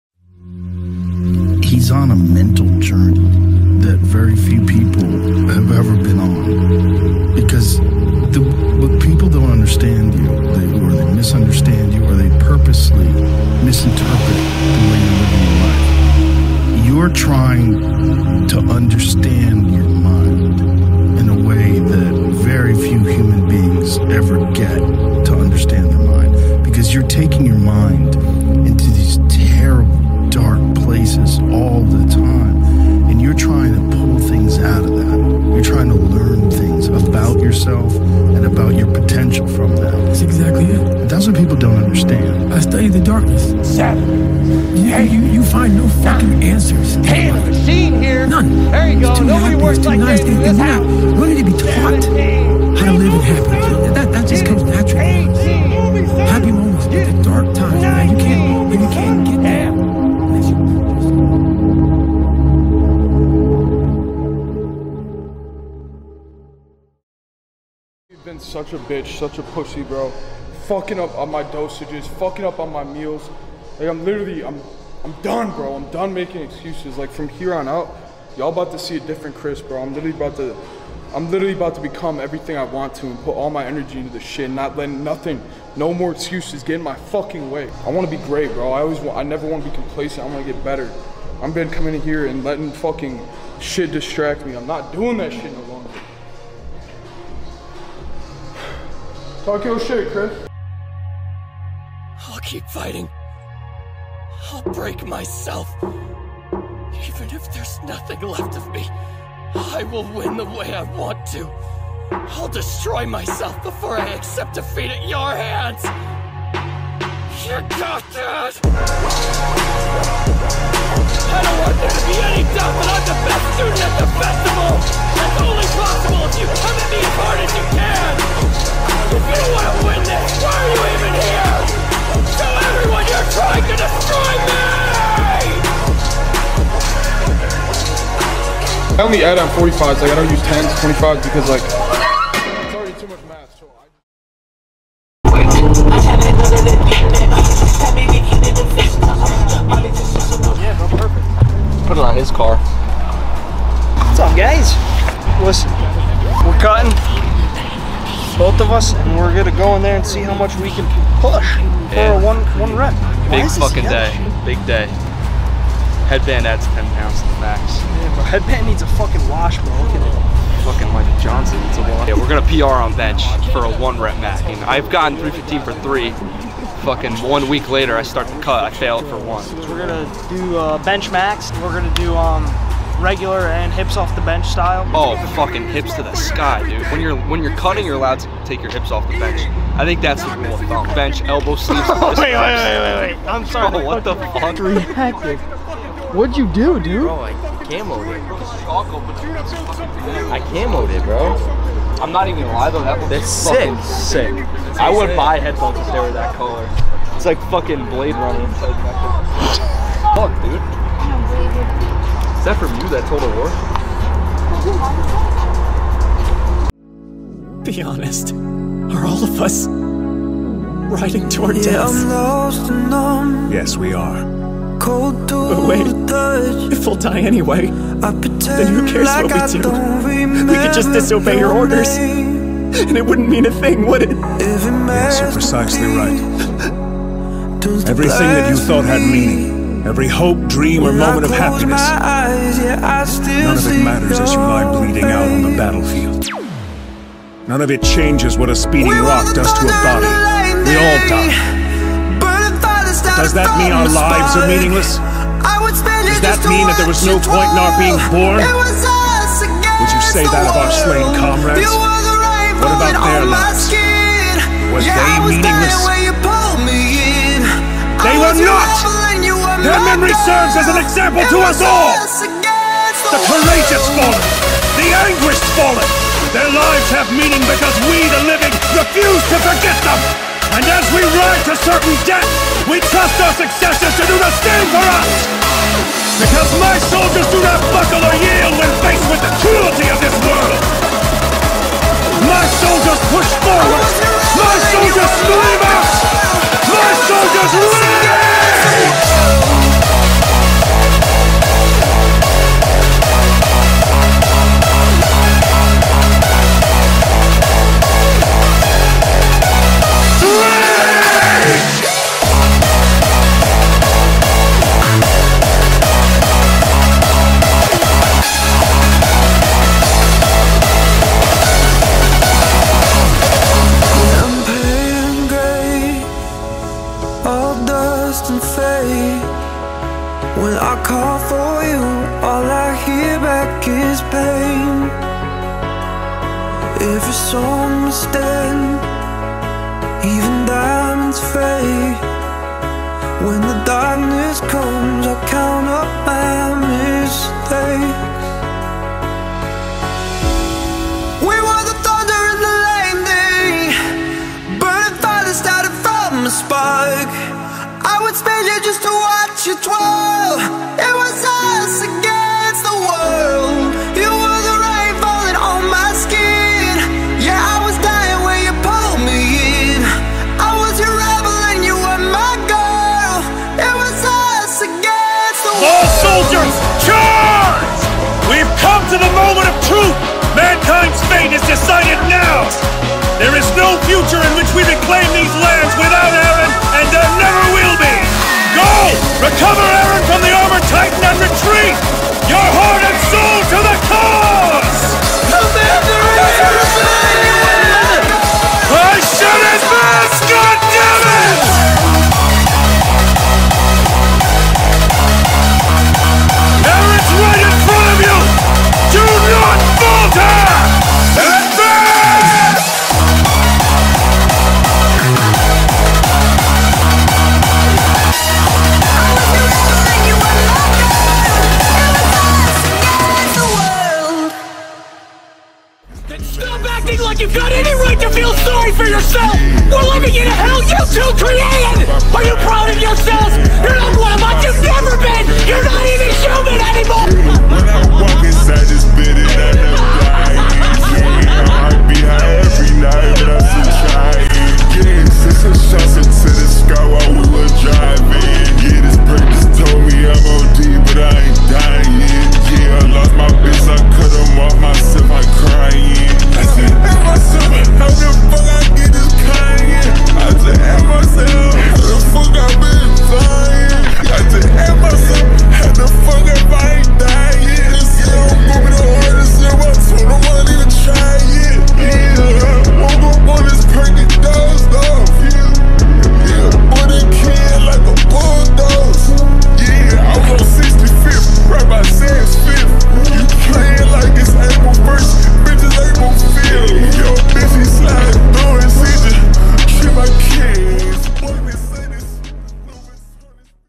REDEMPTION ARC | BENCH PR
after having a loss of motivation Hunt and also Cuso have caught a second wave of motivation (levrone tik Tok's) push day feat. gear

Andrew's Instagram: andrewhunt.__

PJ's Instagram: pj.mancuso